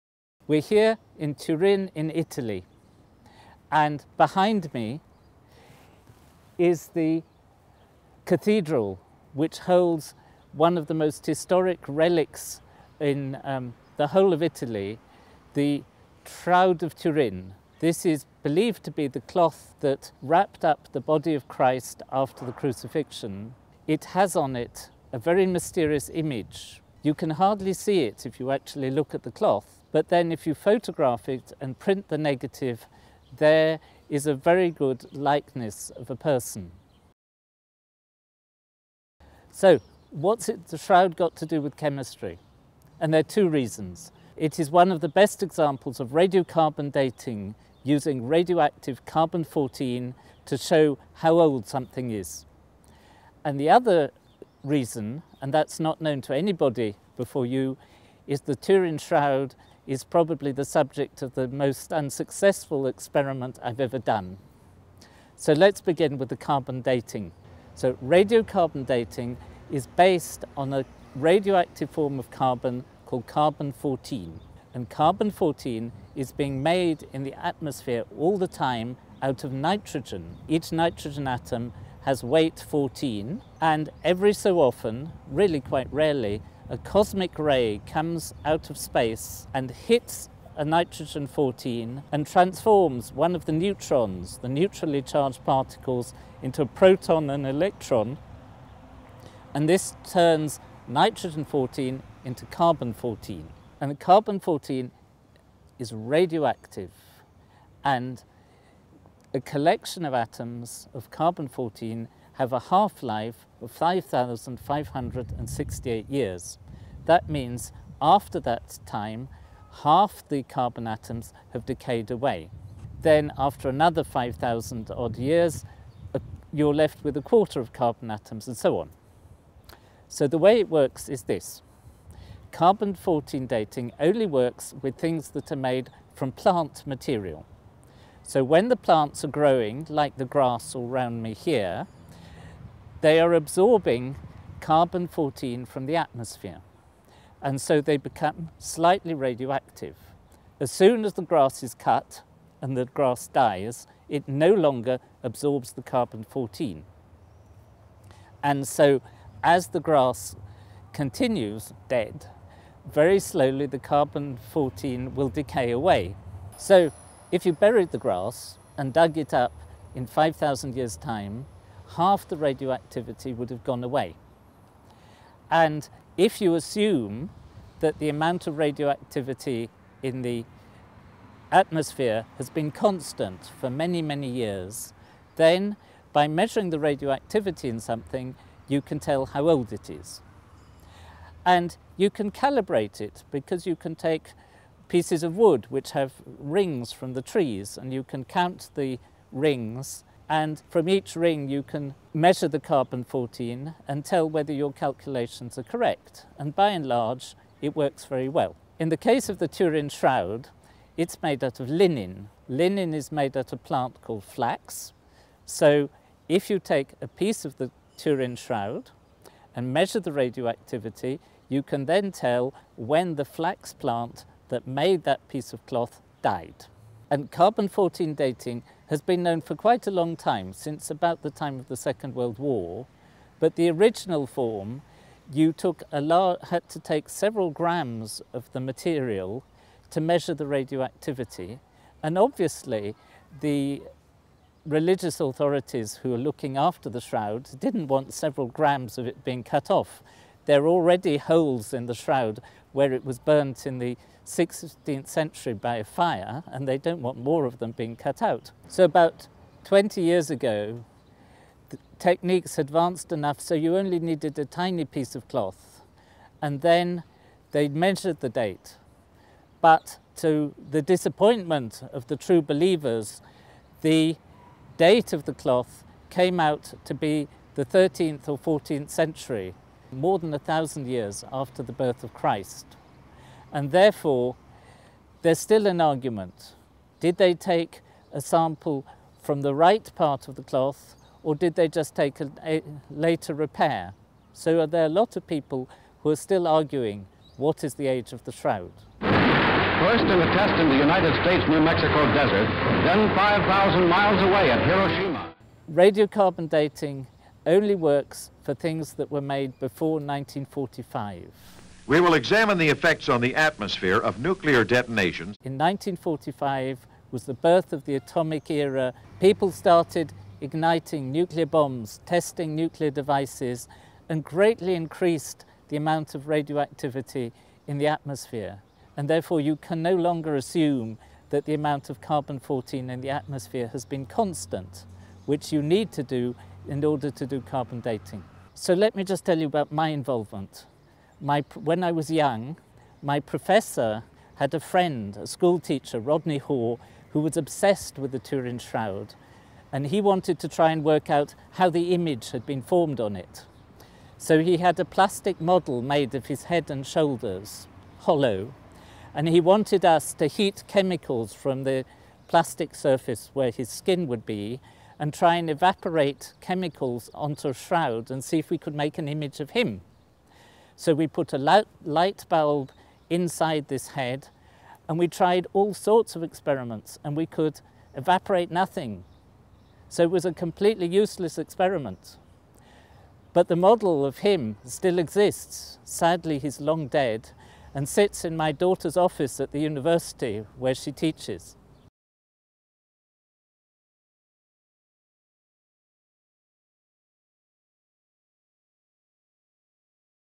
Radiocarbon & Turin Shroud - Periodic Table of Videos
We discuss the famed Turin Shroud in a chemistry context, including how radiocarbon dating has famously been used on the religious relic.

Correction: It's a proton and electron COMBINING to create C14... I made the poor Professor stand in the Italian sun for so long waiting for the right light conditions that I may have temporarily fried his brain. Correction is included as an annotaion.